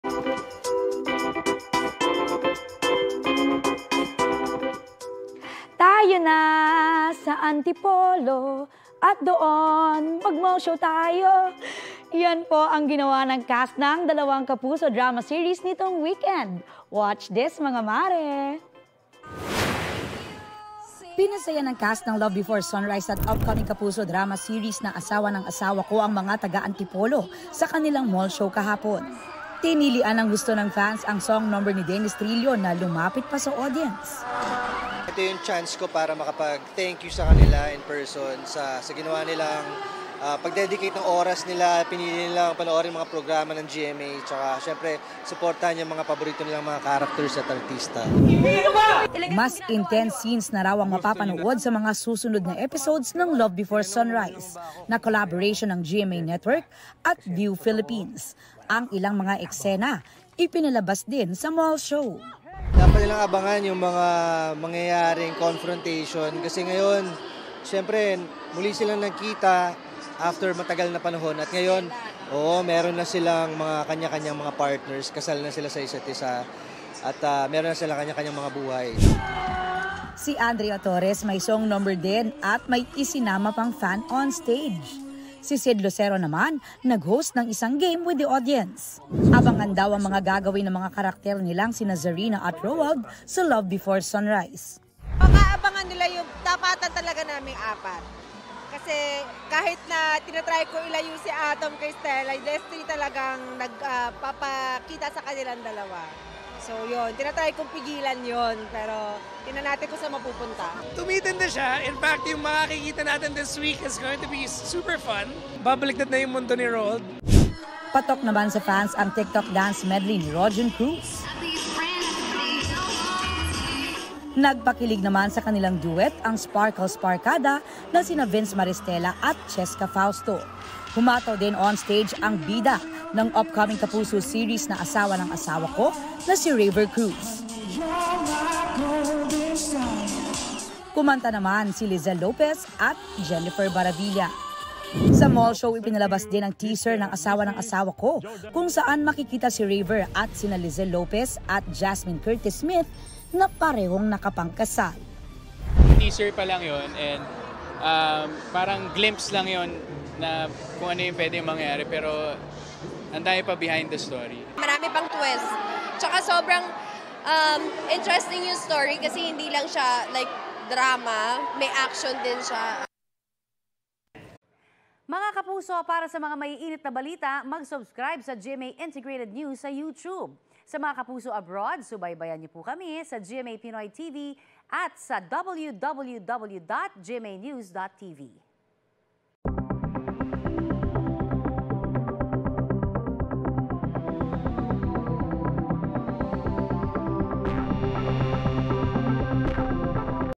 "Love Before Sunrise" at "Asawa ng Asawa Ko" cast members, pinasaya ang mga Kapuso sa isang... | BT
"Love Before Sunrise" at "Asawa ng Asawa Ko" cast members, pinasaya ang mga Kapuso sa isang mall show

Pinasaya ng cast ng "Love Before Sunrise" at upcoming Kapuso Drama series na "Asawa ng Asawa Ko" ang taga Antipolo sa kanilang mall show kahapon